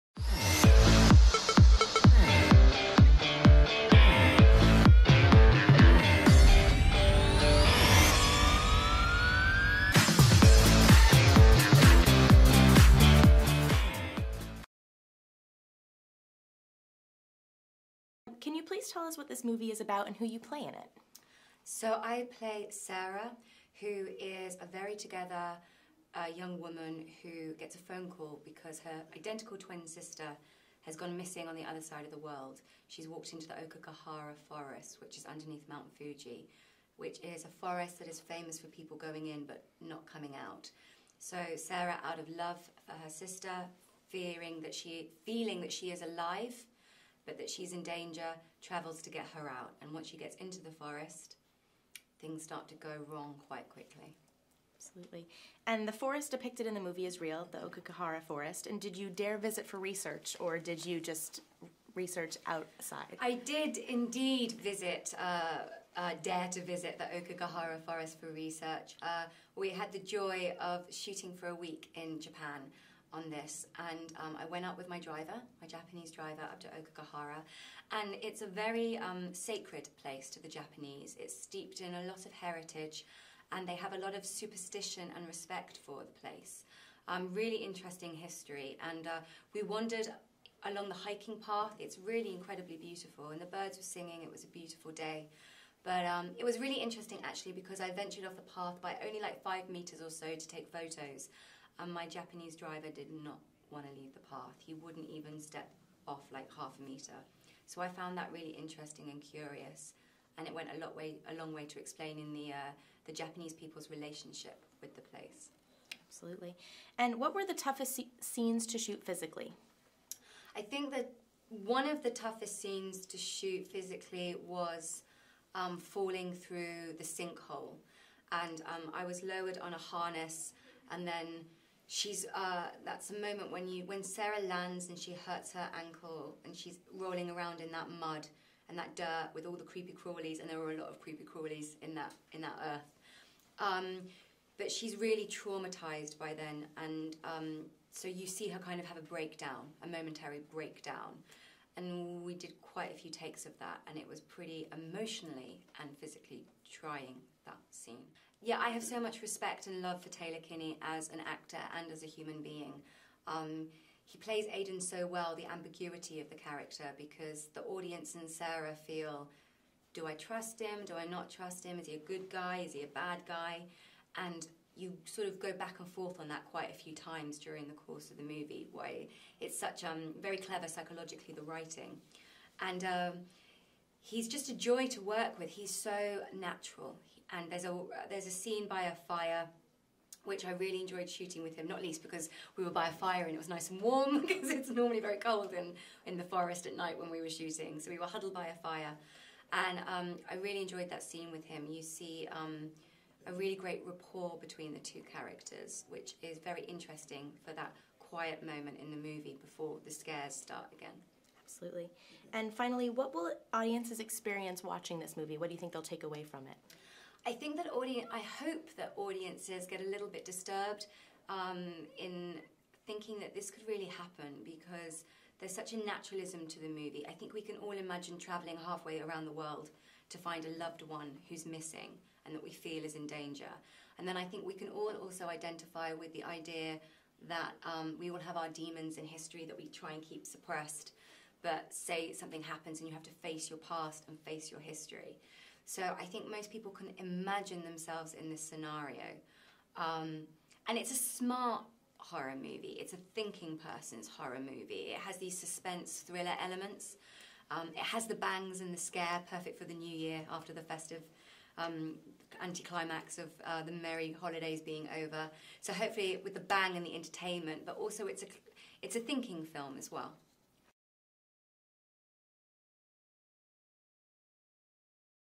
Interview: Natalie Dormer discusses THE FOREST
In this interview of Game of Thrones star Natalie Dormer discusses her latest movie, the horror thriller THE FOREST.